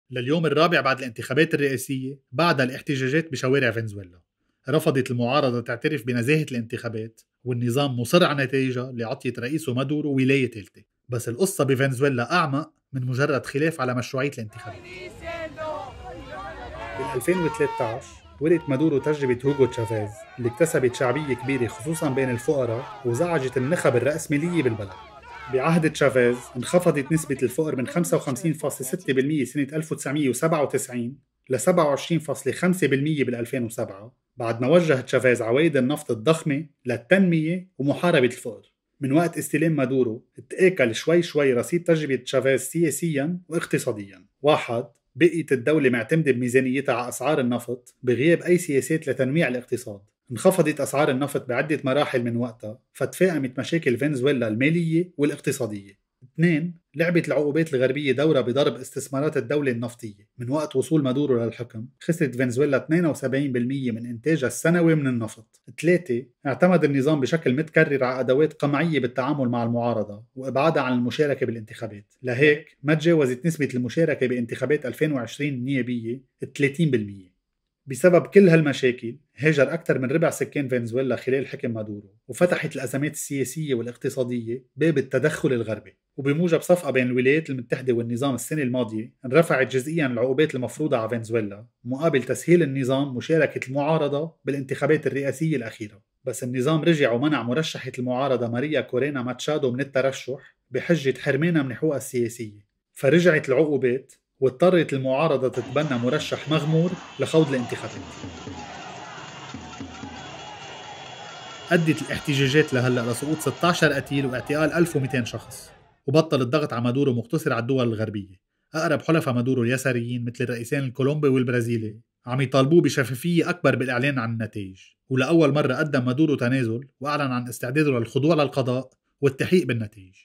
احتجاجات فنزويلا: مادورو يبدّد إرث تشافيز
احتجاجات فنزويلا: كيف تآكل رصيد التجربة التشافيزيّة في عهد مادورو؟